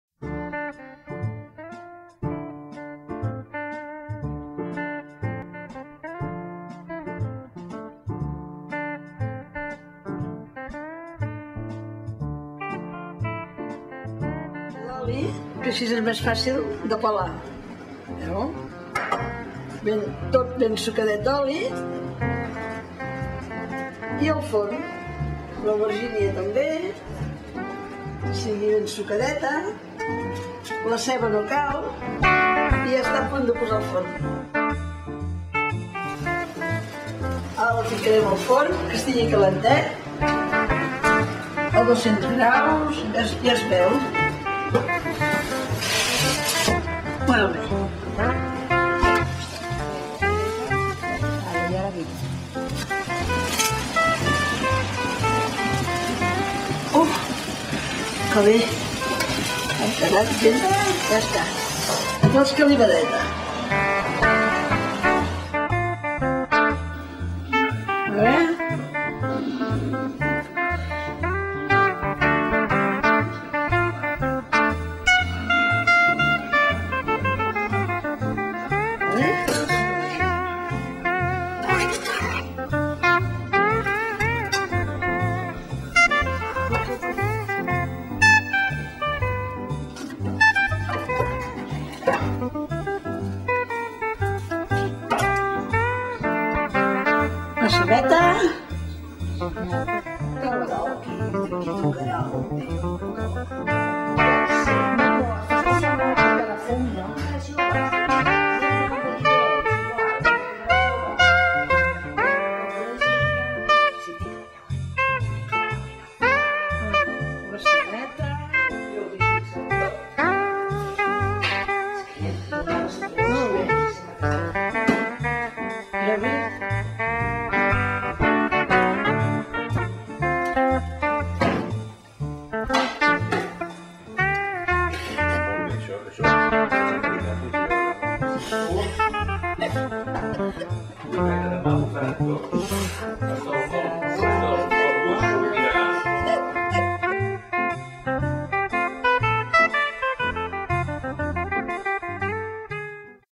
Escalivada
L'escalivada en 2 passos. Explicat per la Titi. CatalanCooking.com